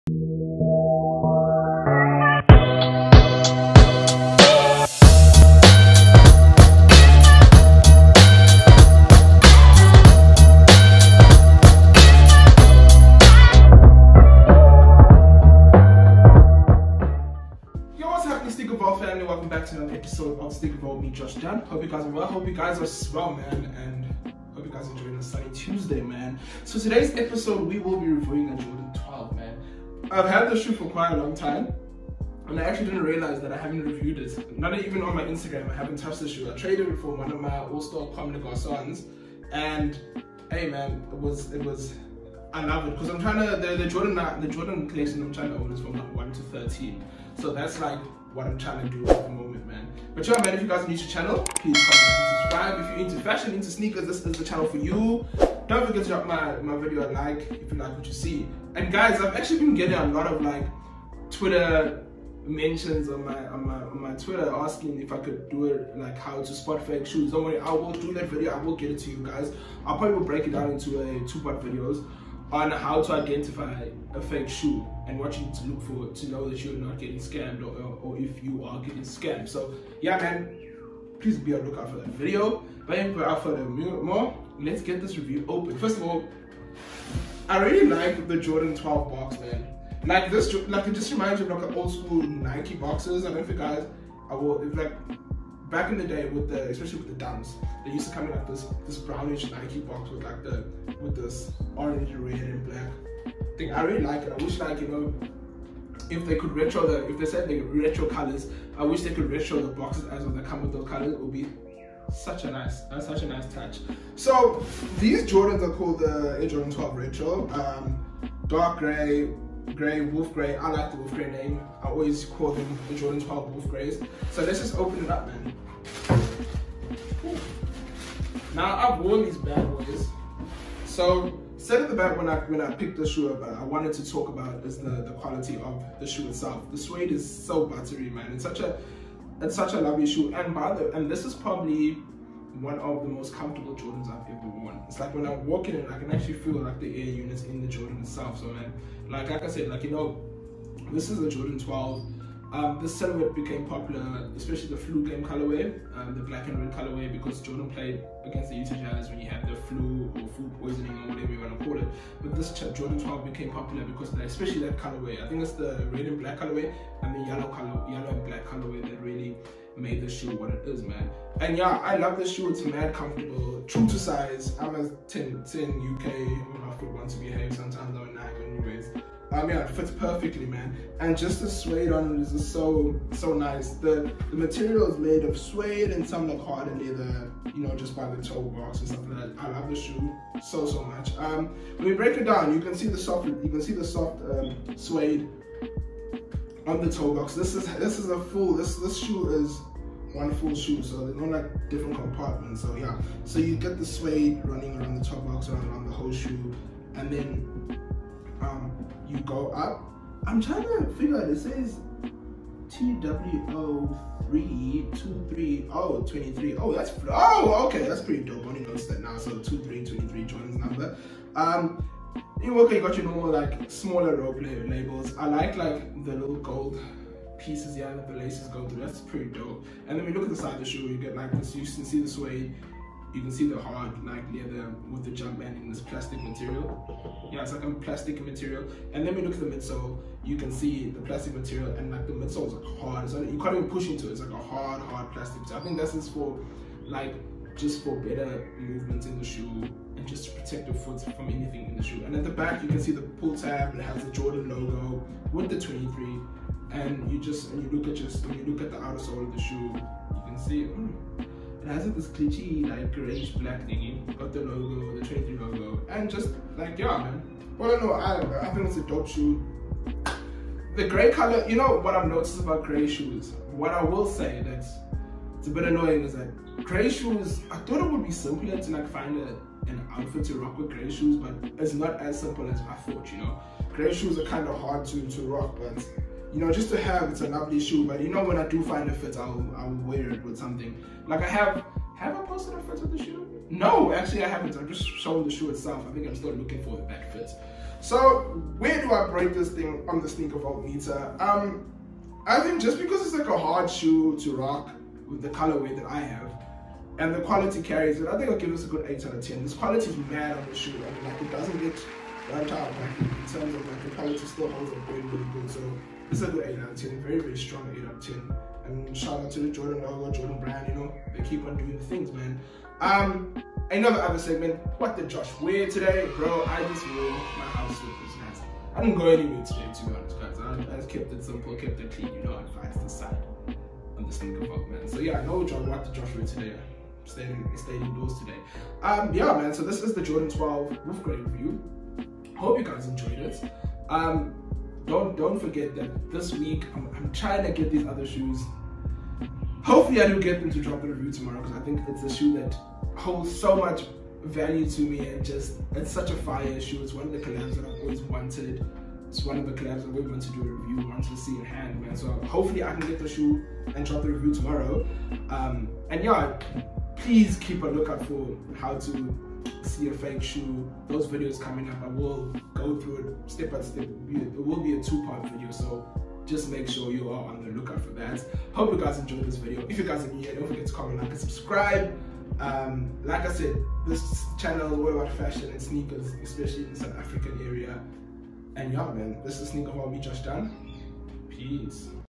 Jordan 12”wolf grey” In-Depth Review| South African YouTuber
This weeks first episode we look at the jordan 12 in the wolf grey colorway